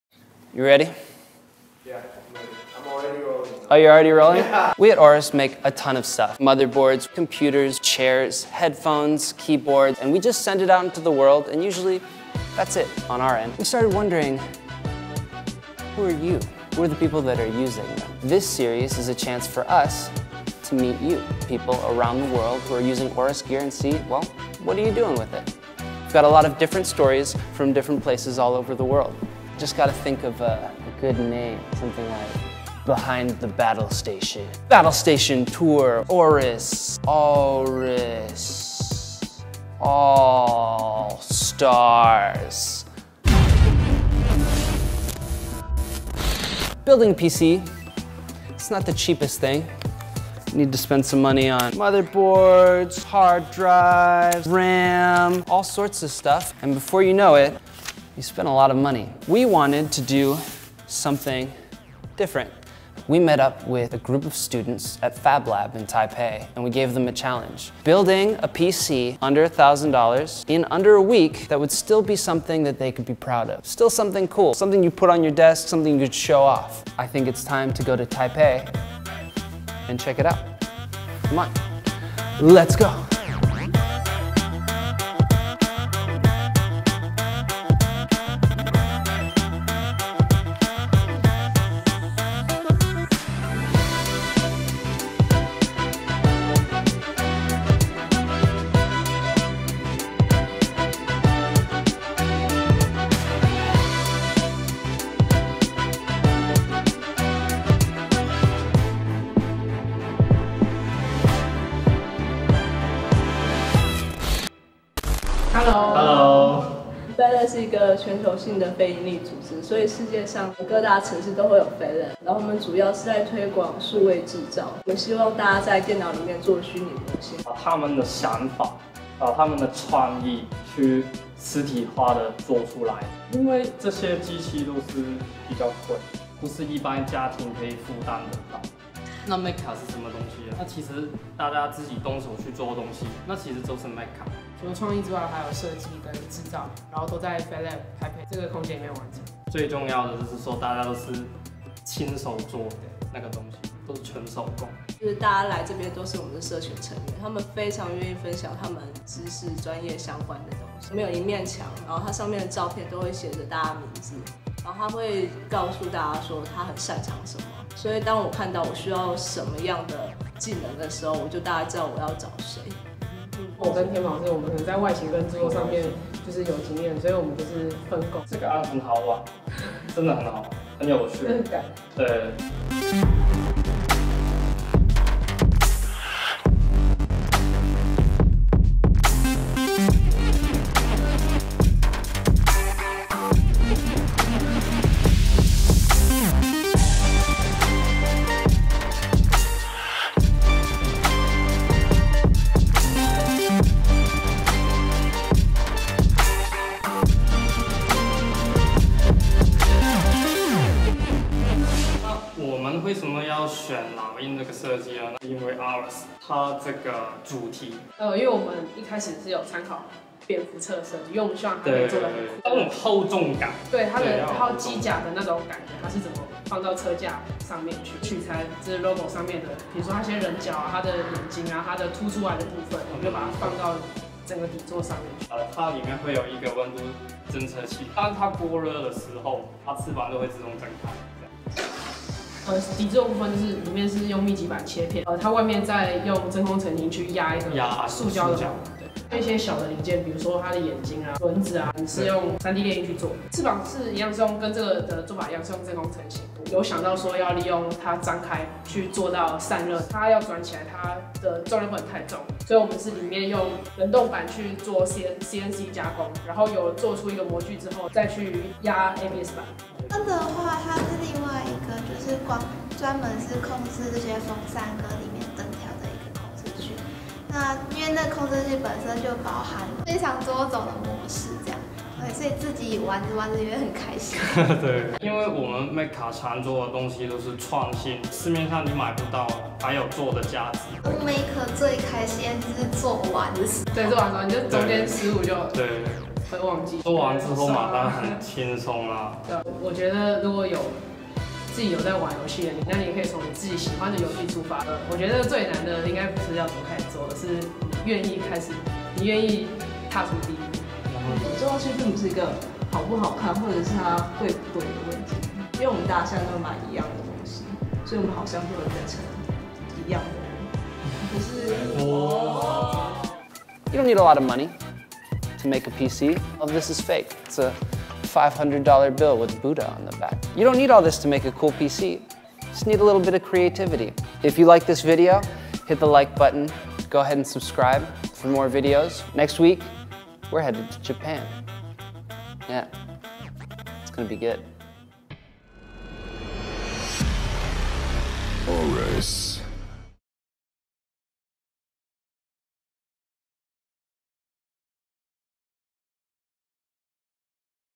THE COOLEST $1K PC EVER MADE | AORUS
We know that when money’s tight, it’s nearly impossible to get a gaming PC. So we teamed up with a group of students at Fab Lab in Taipei to show how with a little creativity and the right tools, you can make something cool and completely one-of-a-kind.